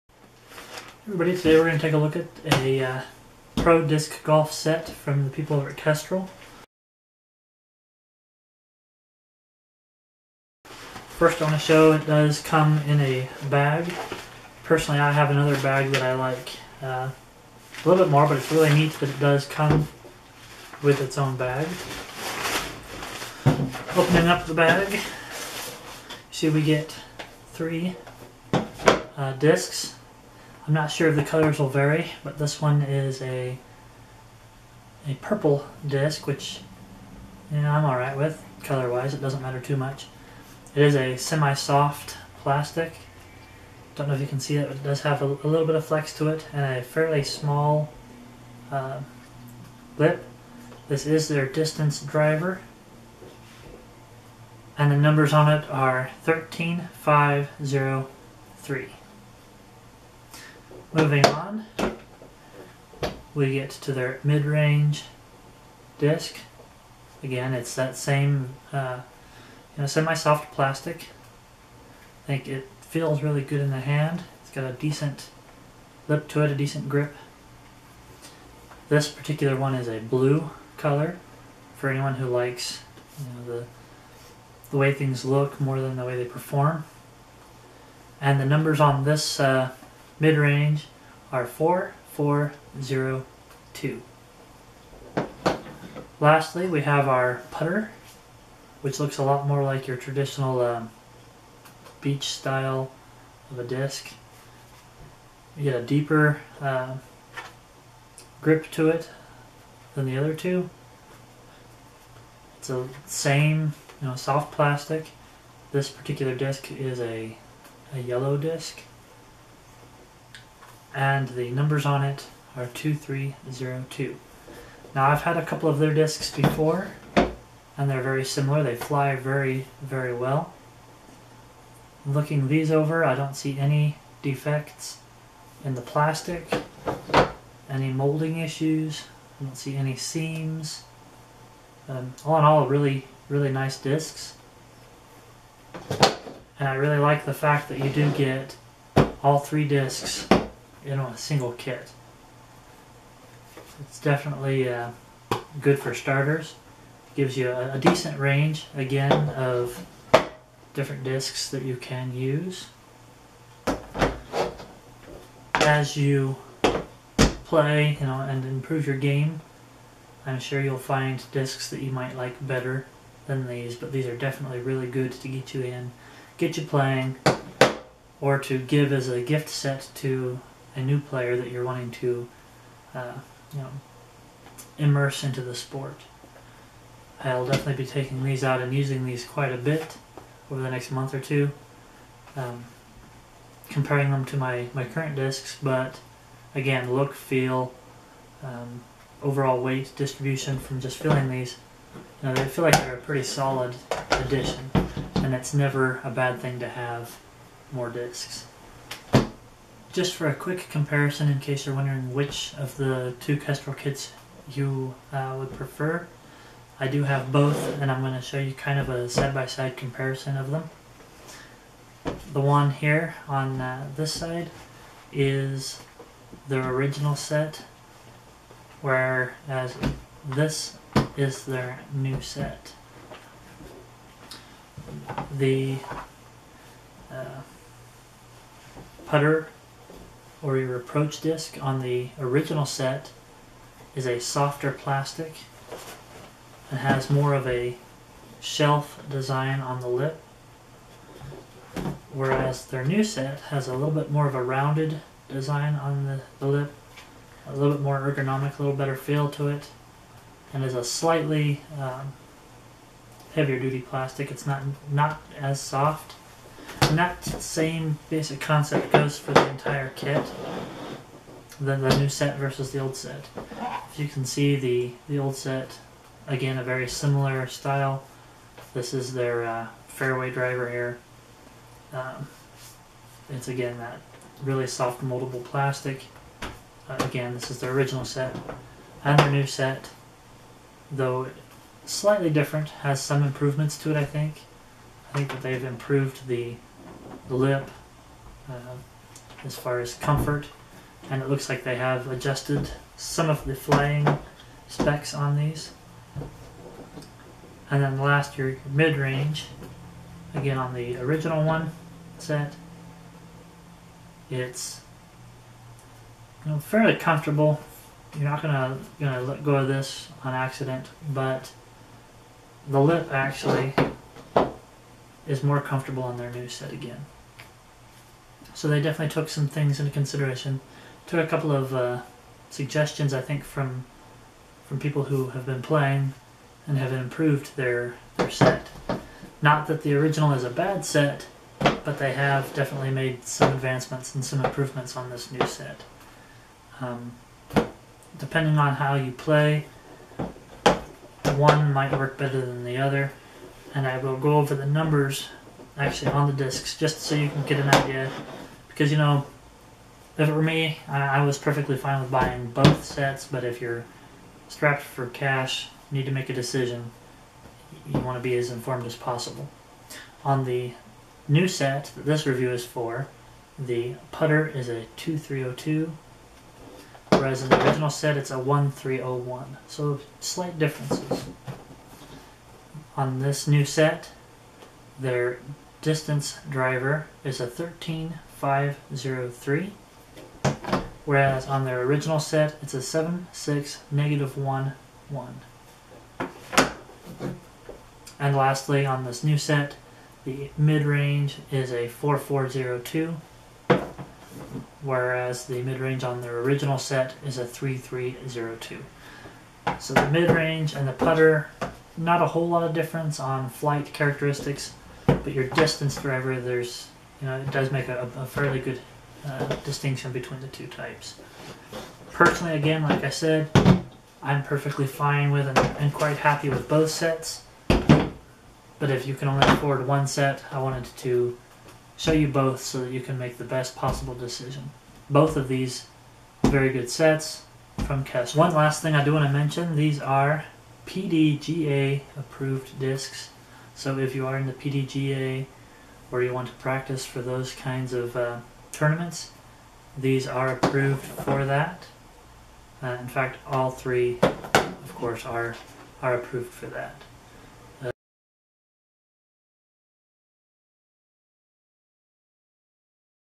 Gear Review: Kestrel Pro Disc Golf Set
Pros:
✅ Great all-in-one set for the beginner, or anyone wanting to suplement their disc collection
✅ Great price per disc
✅ Very comfortable / well weighted discs
✅ PDGA certified
✅ Includes a disc sack to keep the set in.

Cons:
🍅 None

Summary: This is a great starter set to help anyone get into and enjoy the sport of disc golf! It has a nicely balanced, very well thought out selection of discs that perform very well and provide you with everything you need to get out there and start having fun!